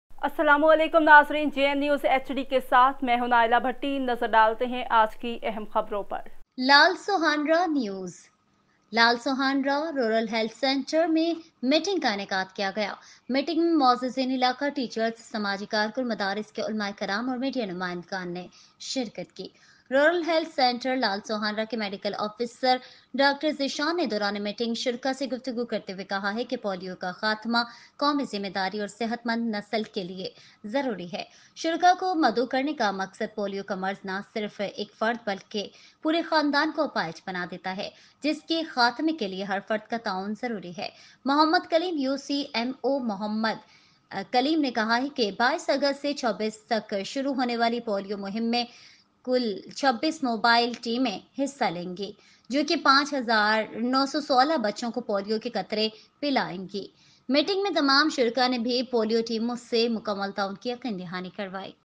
j n news hd Dr. Zeeshan, Medical Officer of Rural Health Center Lal Sohanra, talking to the
Lal Sohanra News
 Osi
 A meeting was held at Lal Suhanra Rural Health Center, the meeting was attended by distinguished local teachers, social workers, Madras scholars and media representatives.

 VO
 Dr. Zeeshan, Medical Officer of Rural Health Center Lal Sohanra, talking to the participants during the meeting said that eradication of polio is necessary for national responsibility and a healthy generation.  In order to eliminate it, the cooperation of every individual is necessary. Muhammad Kaleem UCMO Muhammad Kaleem said that a total of 26 mobile teams will participate in the polio campaign starting from August 22 to 26, which will reach 5916 children.  Polio drops will be administered. All the participants in the meeting also assured full cooperation from the polio teams.